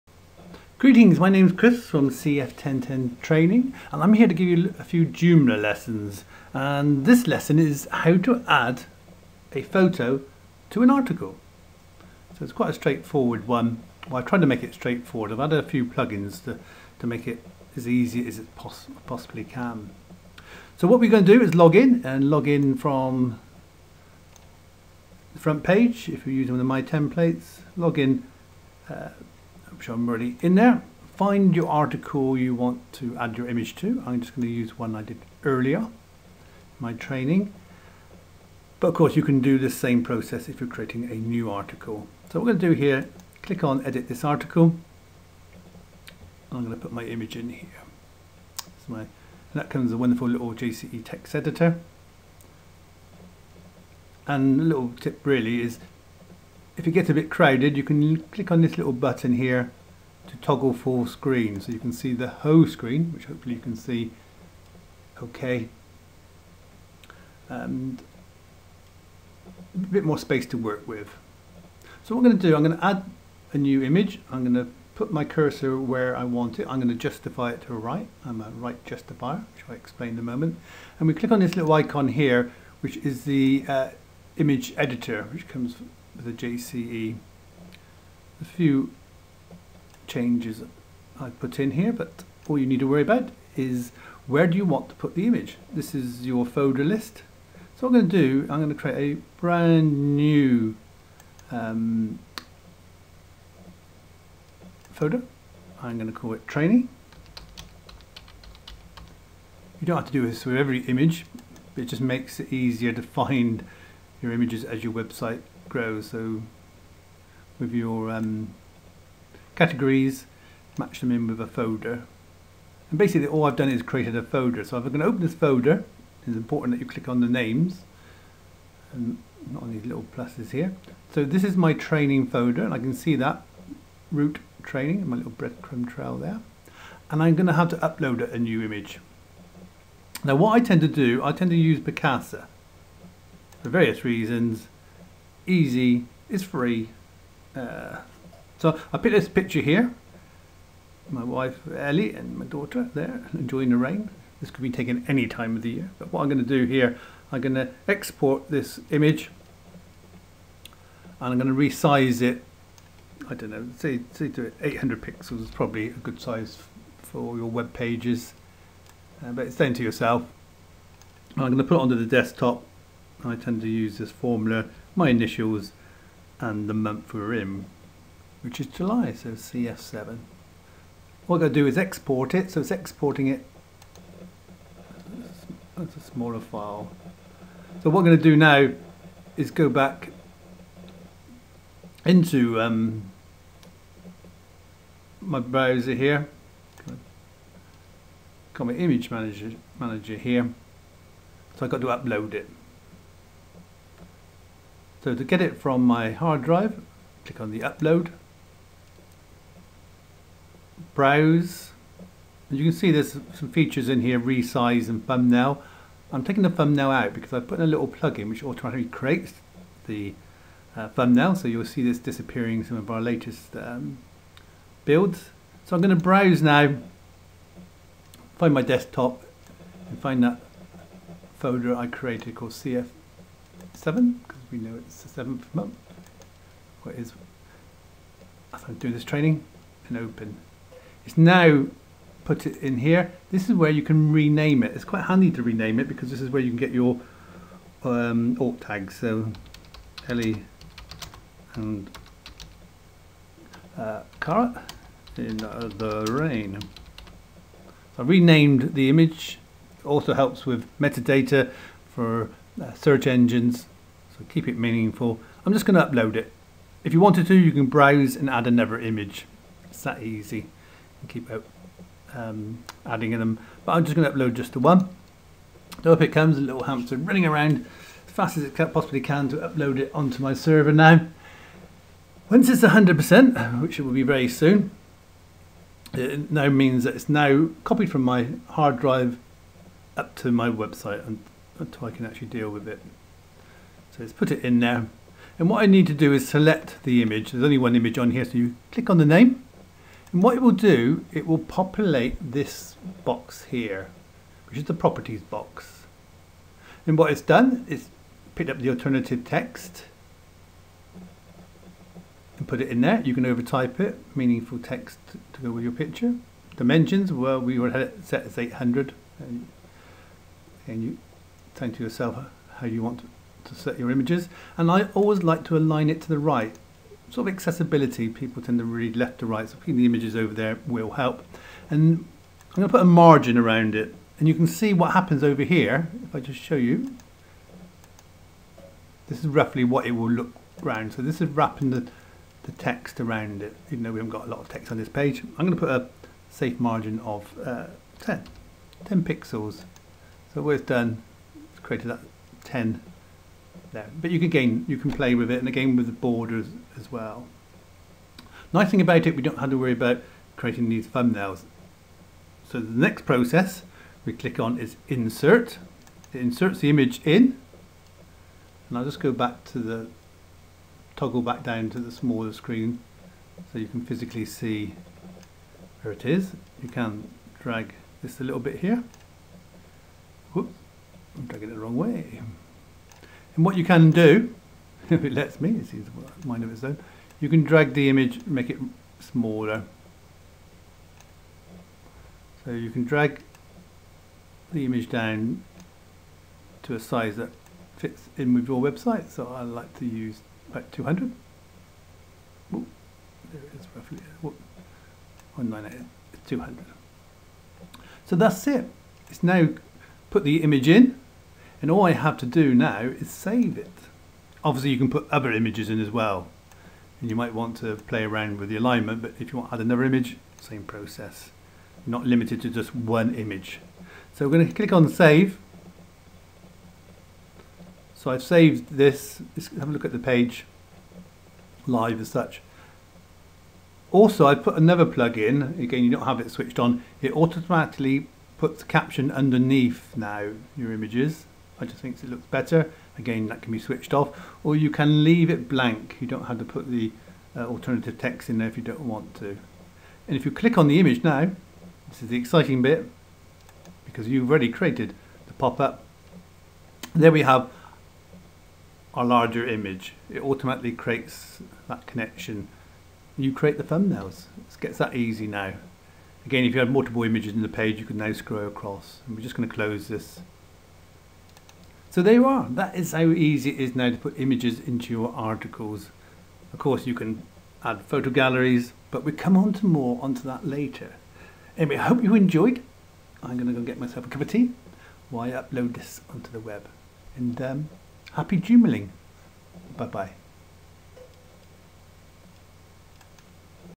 Adding photos to joomla articles
A training video on how to add photos to your cf10ten joomla website demo by chris franklin